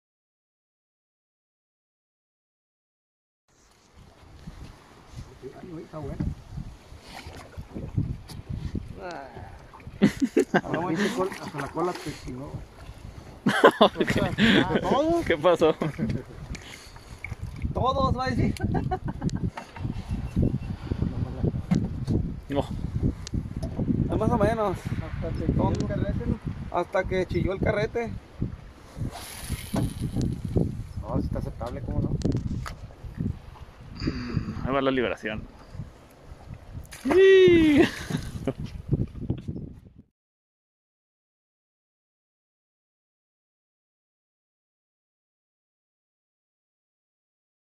Un día de pesca de calicos en Ensenada 28 jun2020
Buen día de pesca en Ensenada... calicos y curvinas... este video solo muestra un buen calico que se fue para atrás para que crezca más...